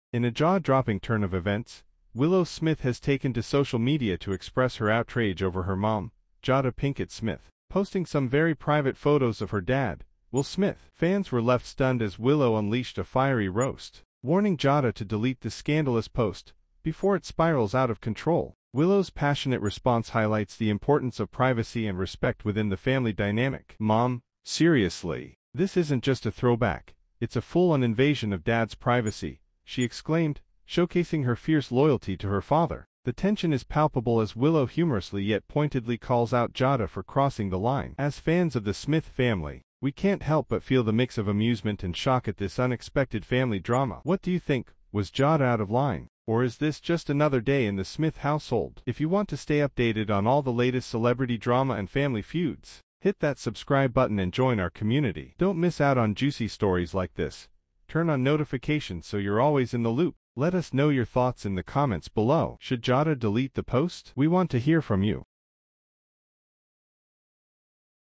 Willow Smith ROASTS Jada for Posting Will's N*des! On Twitter 😱🔥 'DELETE IT NOW!
CelebrityBirthdayAndWillSmith"Willow Smith ROASTS Jada for Posting Will's N*des! On Twitter 😱🔥 'DELETE IT NOW!